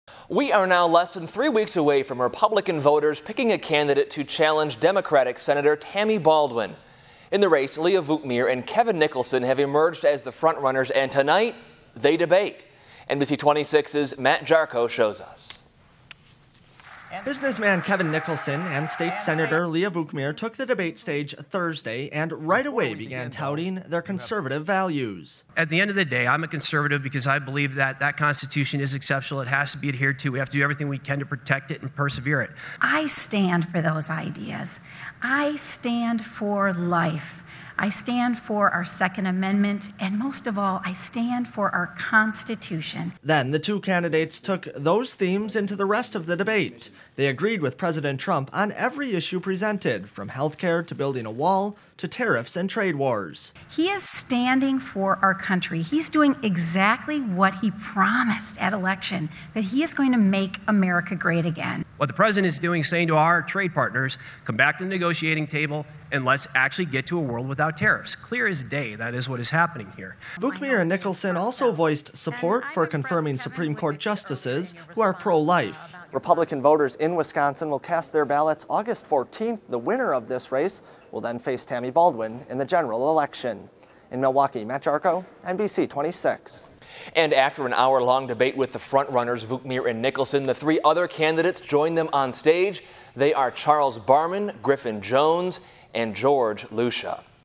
GOP Senate candidates face off in debate
Kevin Nicholson and Leah Vukmir agreed on several issues Thursday night, from conservative values to health care and immigration.